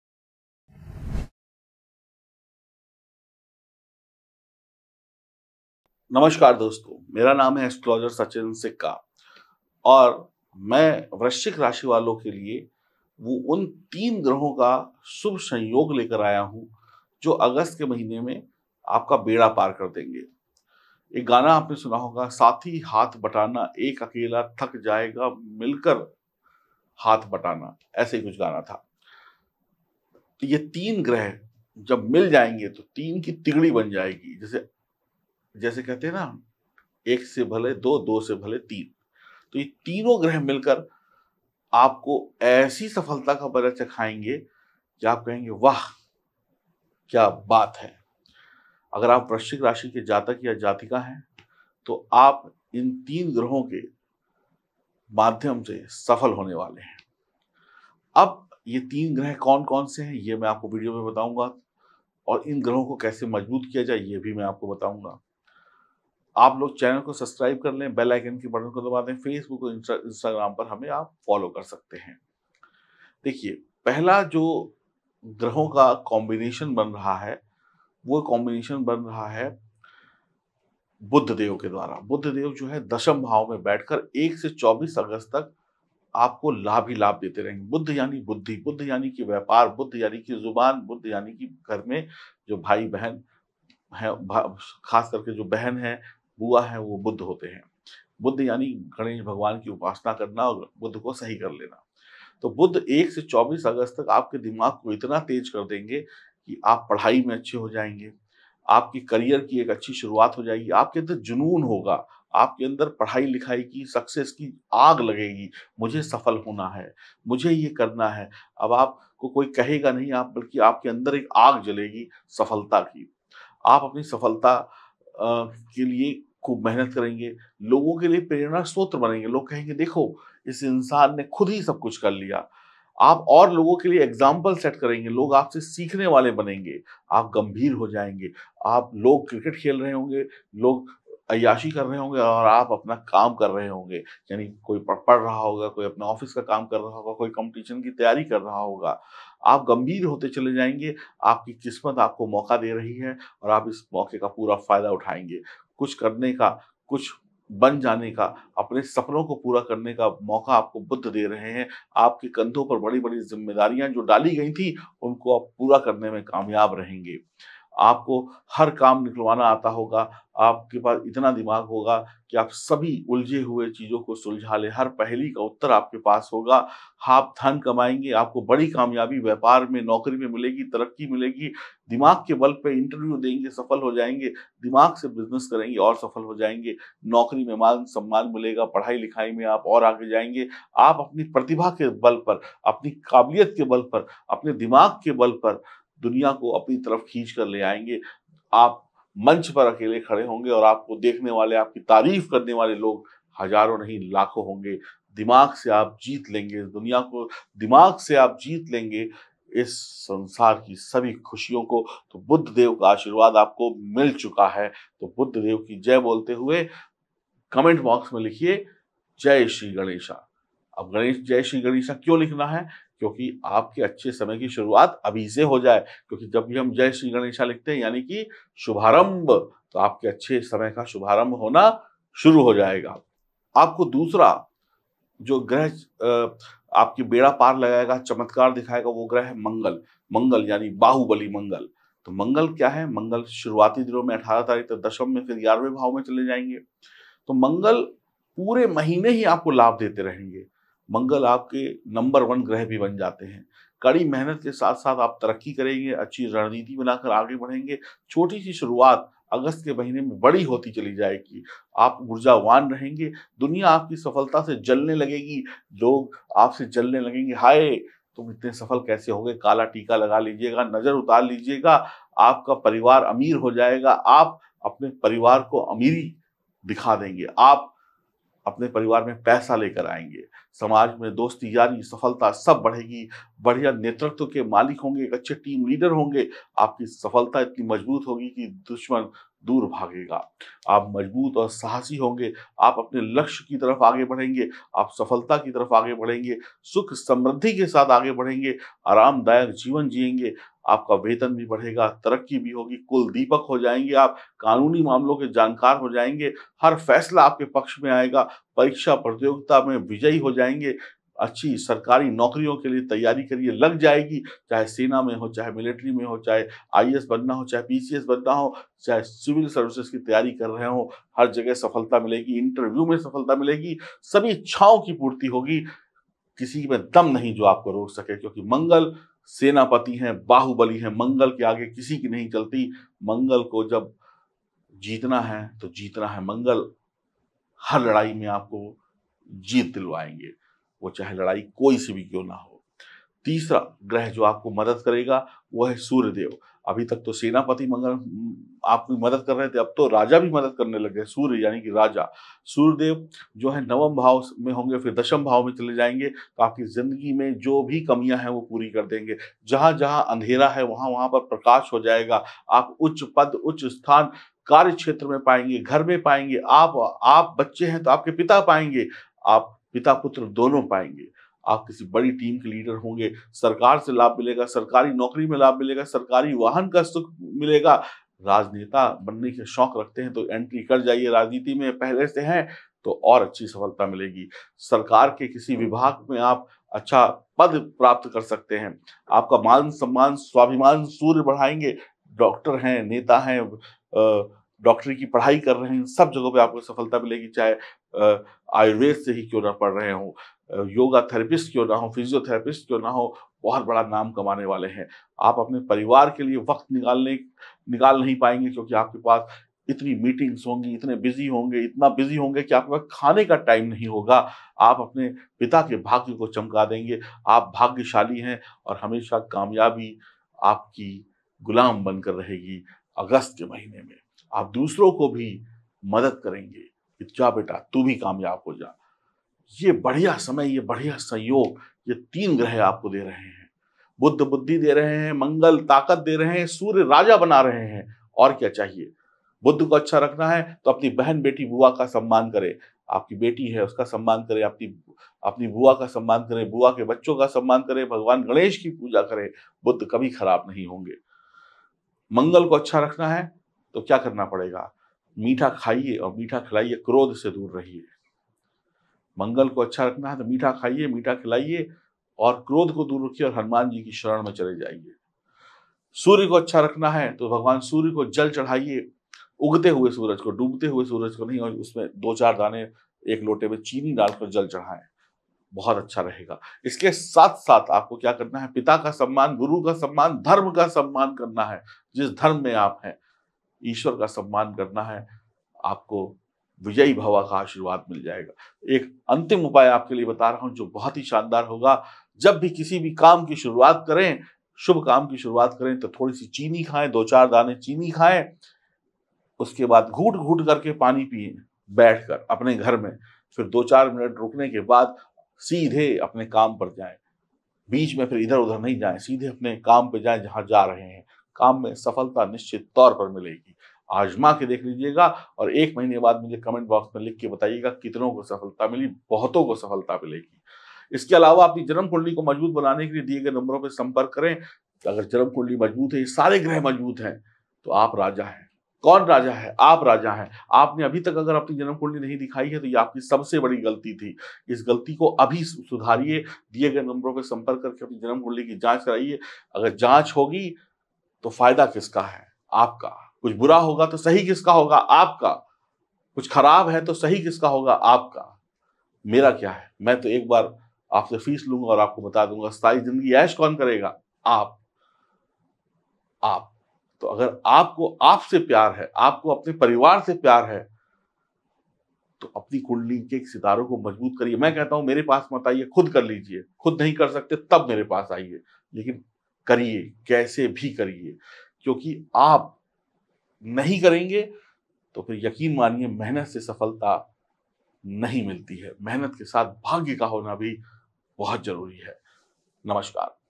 अगस्त के महीने में ये 3 ग्रह बना देंगे वृश्चिक राशि के जातकों को भाग्यशाली।
जानिए कौन से 3 ग्रह बदलने वाले हैं वृश्चिक राशि के जातकों की किस्मत।

कैसा रहेगा वृश्चिक राशि के जातकों के लिए आने वाला अगस्त का महीना।

कैसे होगी वृश्चिक राशि के जातकों पर ईश्वर की कृपा।

किन उपायों को करने से हो जाएँगी आपकी सभी समस्याएं ख़त्म।

जानिए सभी सवालों के जवाब ज्योतिषाचार्य सचिन सिक्का जी के साथ।